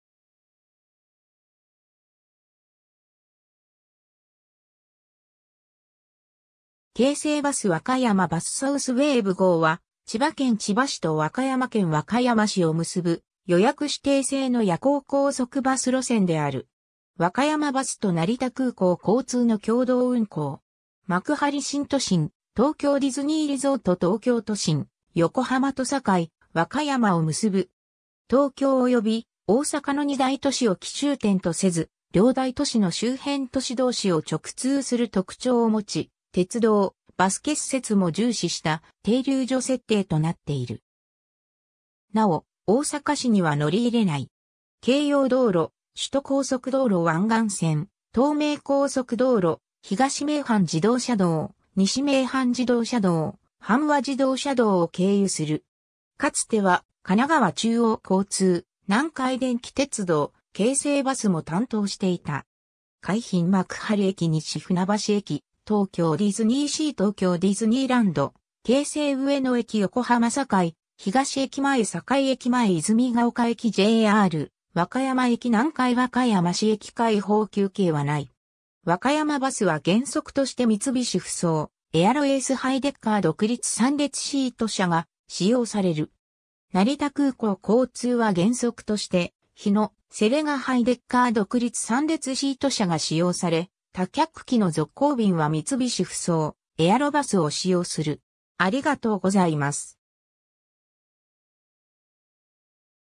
サウスウェーブ号
京成バス（現在の車両、共通運用のファンタジアなごや号） 和歌山バス（現在の車両） サウスウェーブ号（サウスウェーブごう）は、千葉県千葉市と和歌山県和歌山市を結ぶ予約指定制の夜行高速バス路線である。
和歌山バスと成田空港交通の共同運行。
 幕張新都心・東京ディズニーリゾート (TDR) ・東京都心・横浜と堺・和歌山を結ぶ。
東京及び大阪の2大都市を起終点とせず、両大都市の周辺都市同士を直通する特徴を持ち、鉄道・バス結節も重視した停留所設定となっている。
なお大阪市には乗り入れない。
京葉道路・首都高速道路湾岸線・東名高速道路・東名阪自動車道・西名阪自動車道・阪和自動車道を経由する。
 かつては神奈川中央交通 （横浜 - 和歌山系統）、南海電気鉄道 （千葉 - 和歌山系統）、京成バス（子会社の成田空港交通に移管）も担当していた。
 海浜幕張駅 - 西船橋駅 - 東京ディズニーシー - 東京ディズニーランド - 京成上野駅 - 横浜 (YCAT) ⇔ 堺東駅前 - 堺駅前 - 泉ヶ丘駅 - JR和歌山駅 - 南海和歌山市駅 開放休憩（乗客も車外に出られる休憩）はない（SA・PAでの停車は乗務員交代と車両点検のみ）。
 和歌山バスは原則として三菱ふそう・エアロエースハイデッカー独立3列シート車が使用される。
成田空港交通は原則として日野・セレガハイデッカー独立3列シート車が使用され、多客期の続行便は三菱ふそう・エアロバスを使用する。